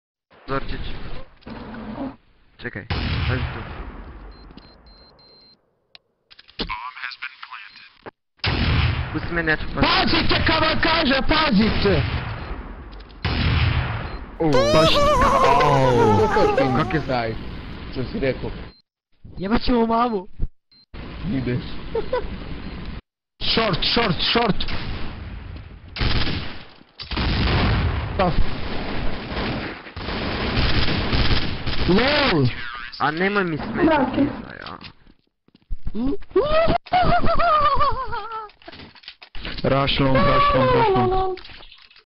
claimb and kinny goes MAD! :DD ahahahaha [DESC.]
THIS IS NOT FAKED, THIS IS PLAYED ON REAL CLANWAR.. DEMO IS OLD.. AND I SERIOUSLY MEAN THAT, IS FREE ONLY FOR GHOST,SUNNY AND SPARKLES MEMBERS ;) HF LIMIIIITEDDD, CLAIMMMBBB, KINNYYYY.. and rest of team suck. :DDDD